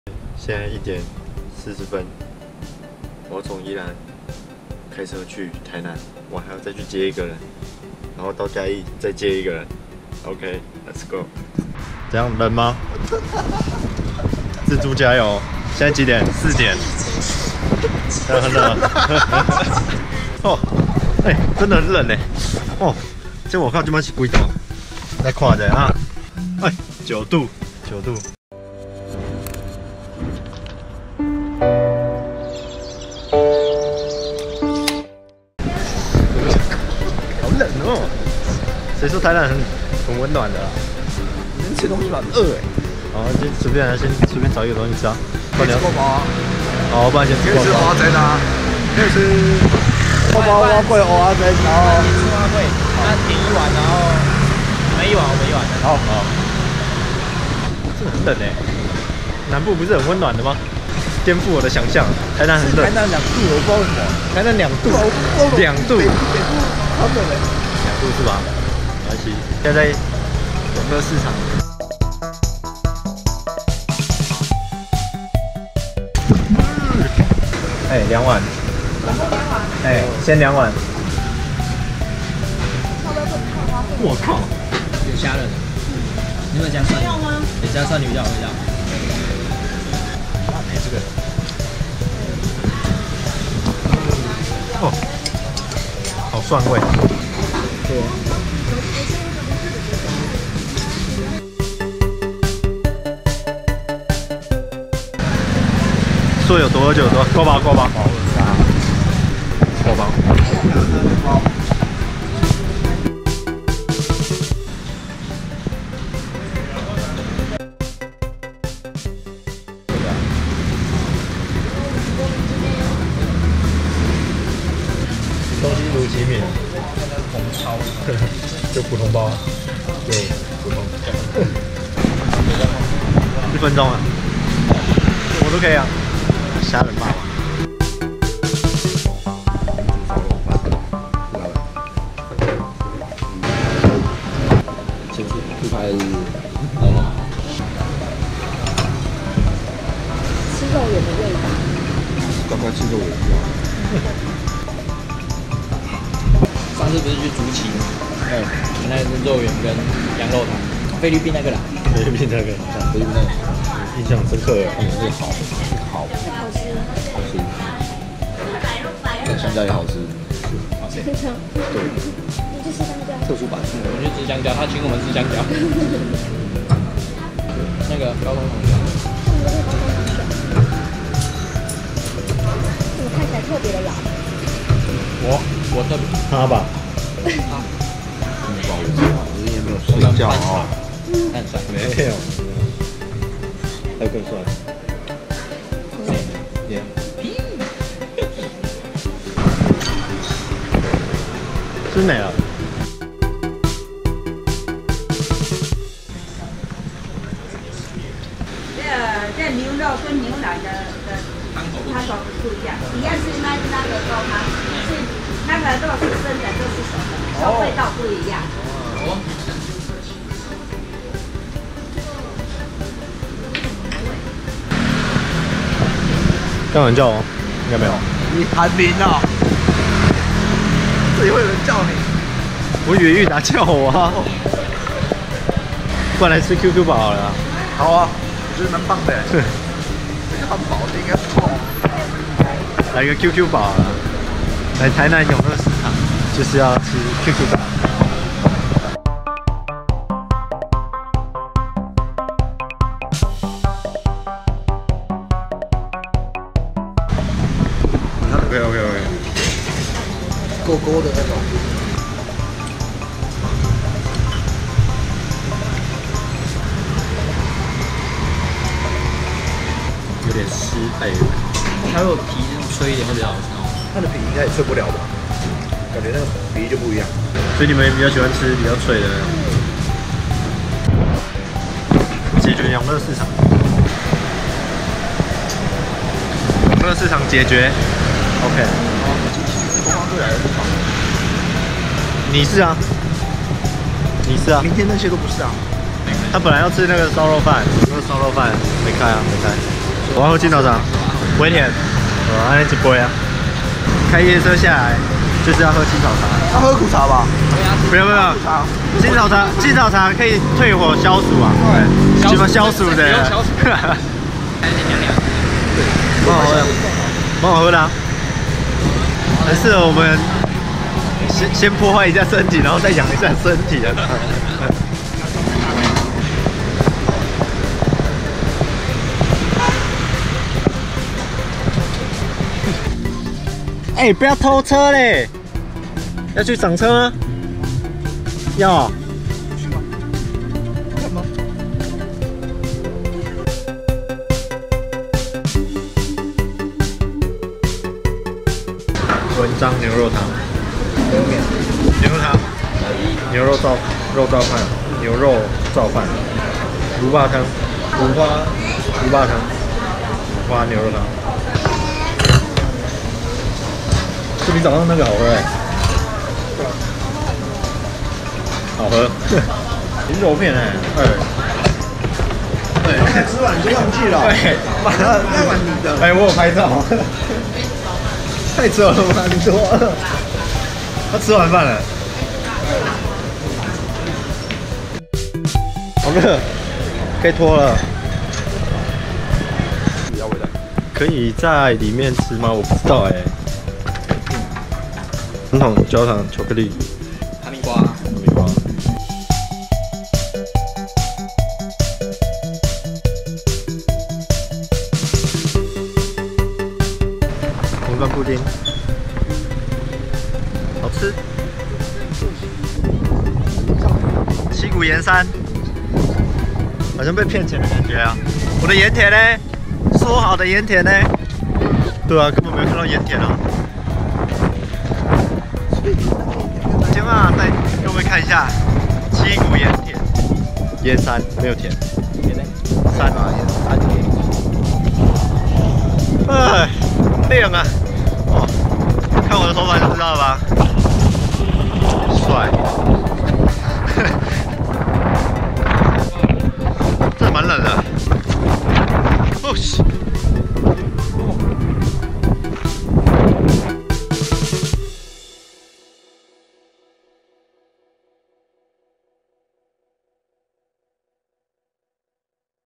【玩樂Vlog】台南吃貨之旅Part.1 到了鹽山竟然找不到鹽田?
人生二訪台南這次行程主要都放在吃
這次分集剪輯讓大家看得比較清楚
從永樂市場開啟台南的行程
讓我帶大家走一圈台南
嗯..差不多是觀光客走的行程
沒關係反正我本來就對台南不熟悉😁✌️